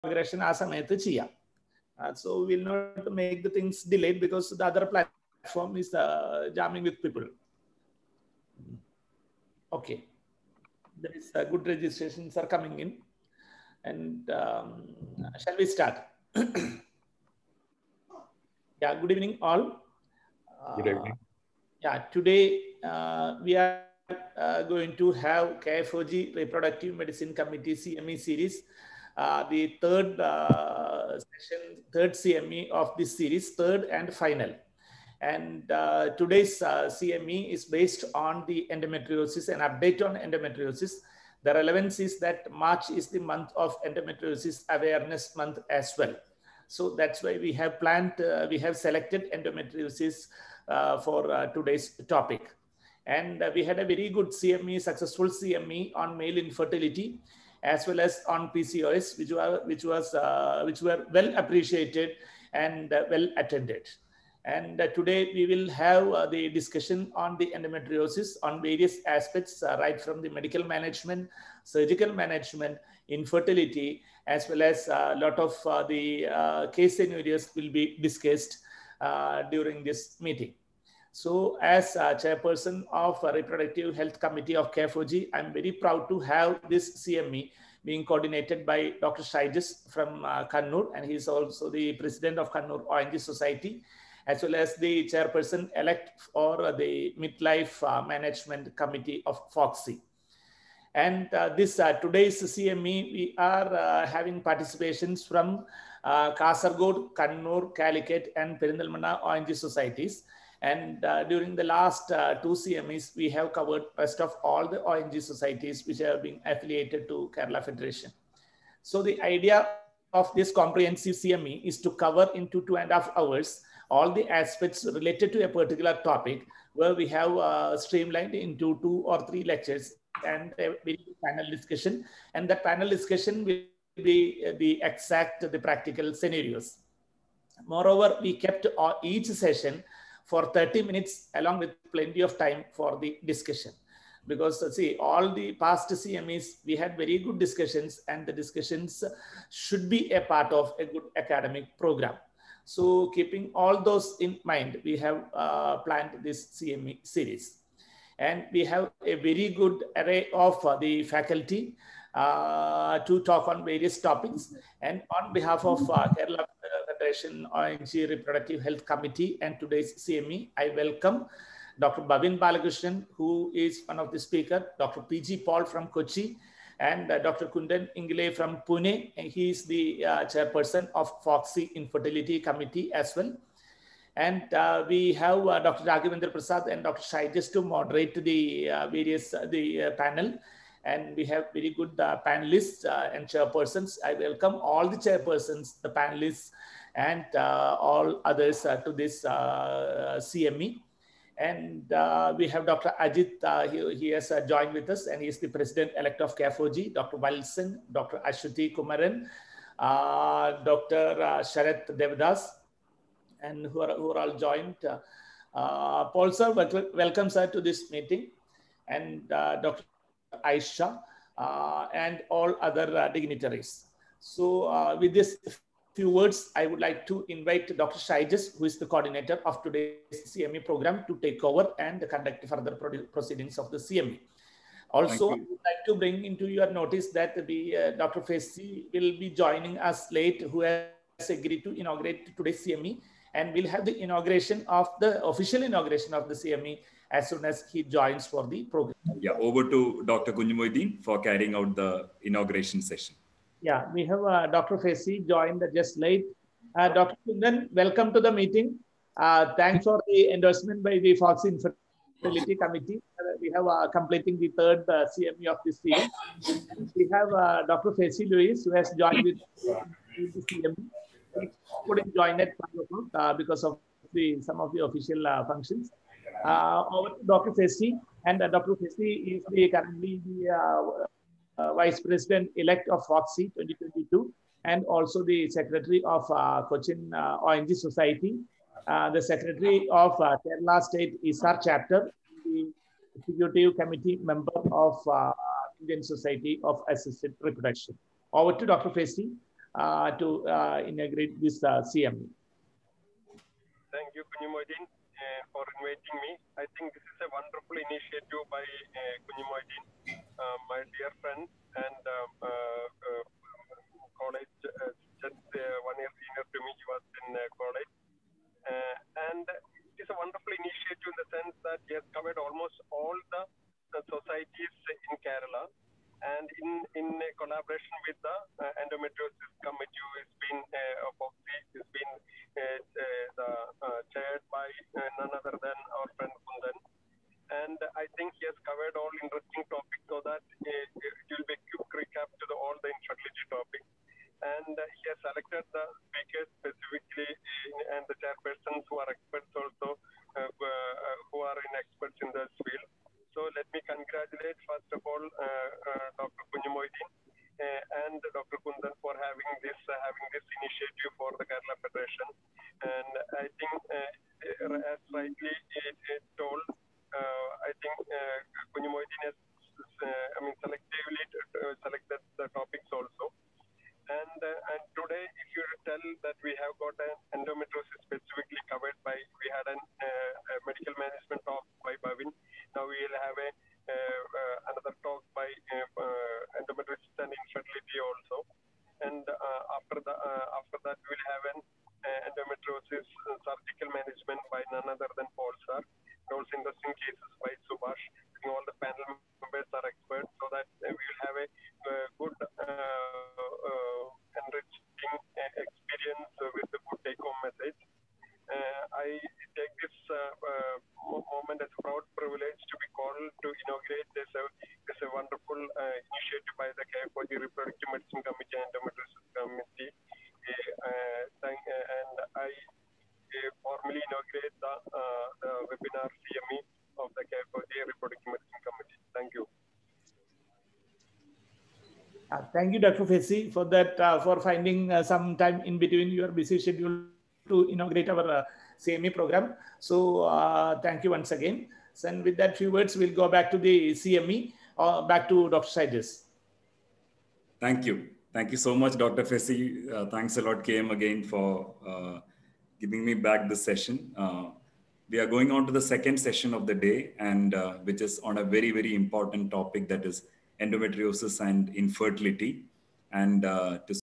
INAUGURATION KFOG RHC CME ENDOMETRIOSIS
KFOG REPRODUCTIVE HEALTH COMMITTEE CME ON ENDOMETRIOSIS- INAUGURATION BY DR FESSY LOUIS